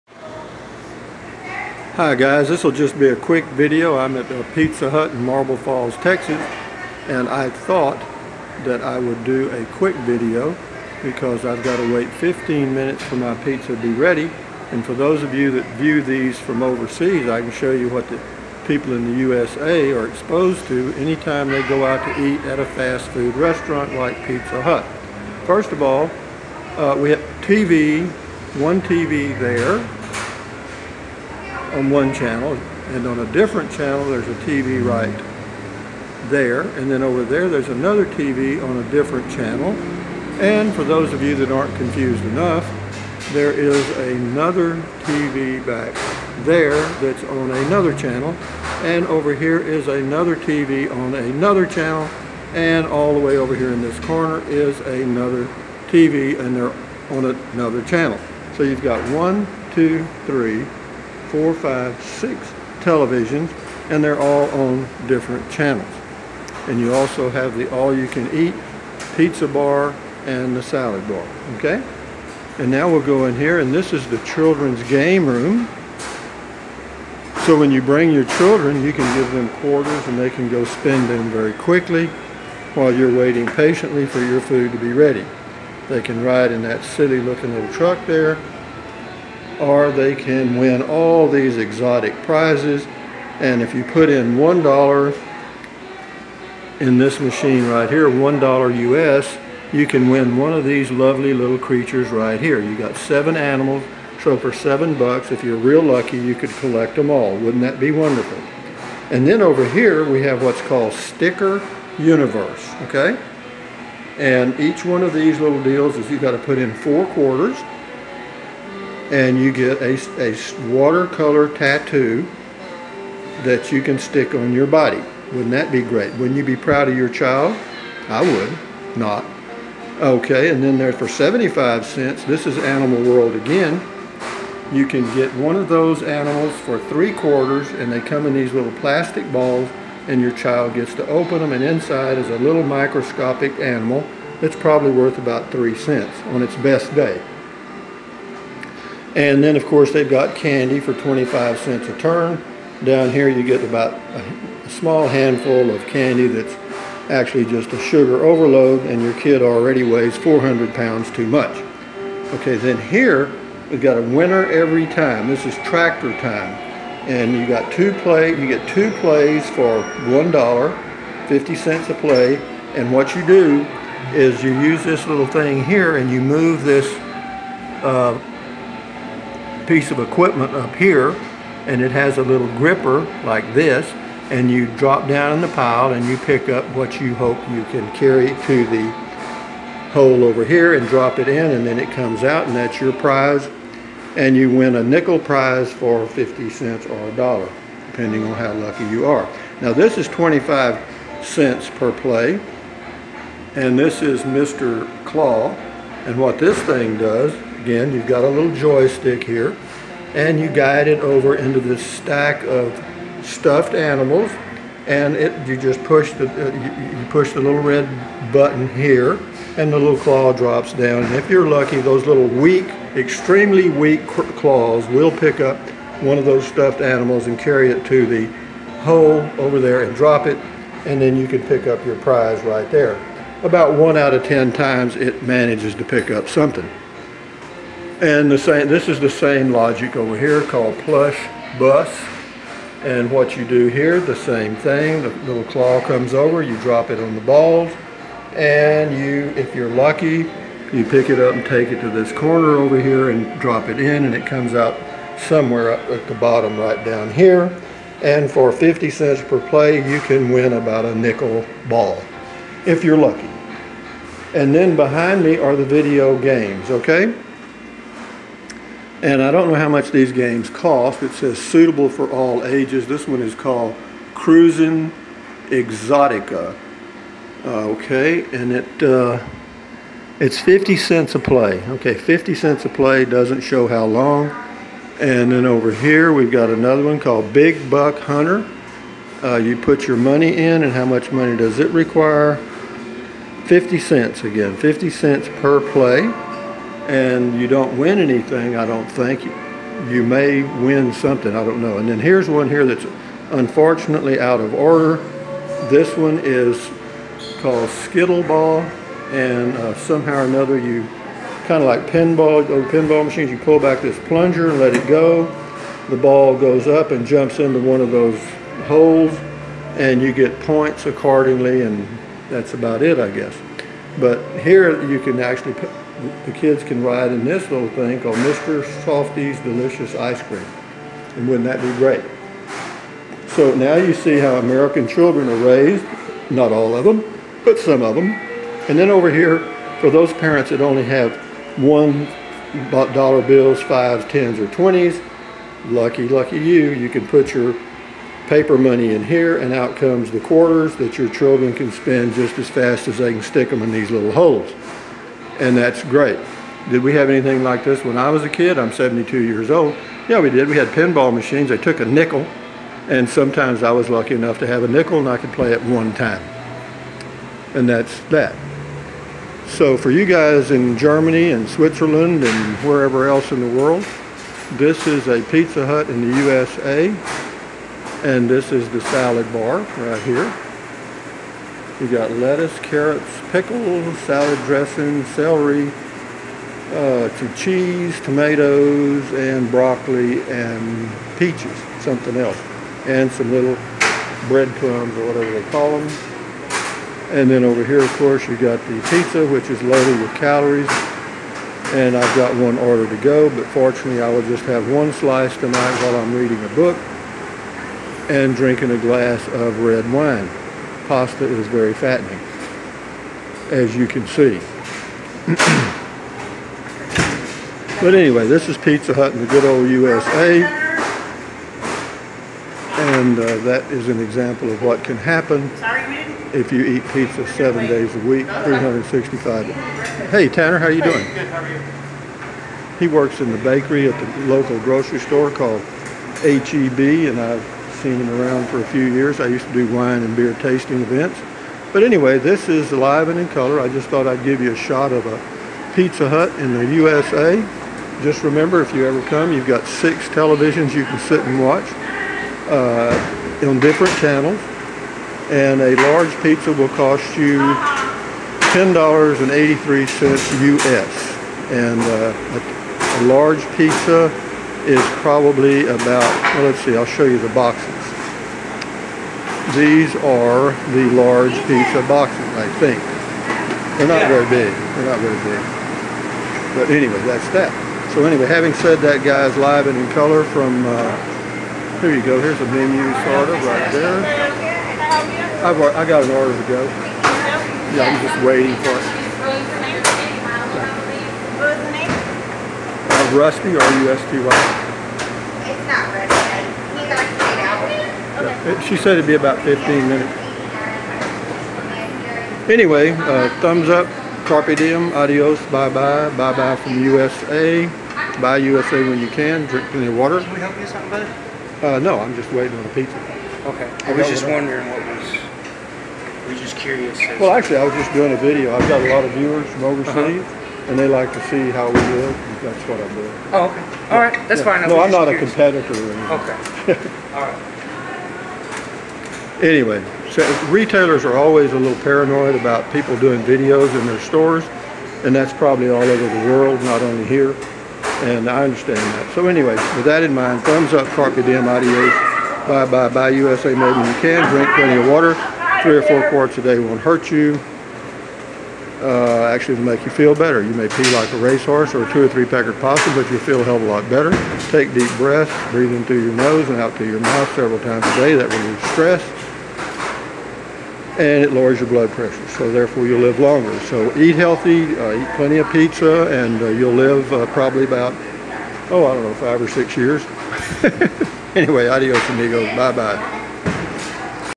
Live from a Pizza Hut in Central Texas USA
rusty78609...While waiting on my to go order I did this short video of life in the USA today. you can contact me by mail to: Rusty, POB 1446, Kingsland Tx or PayPal to !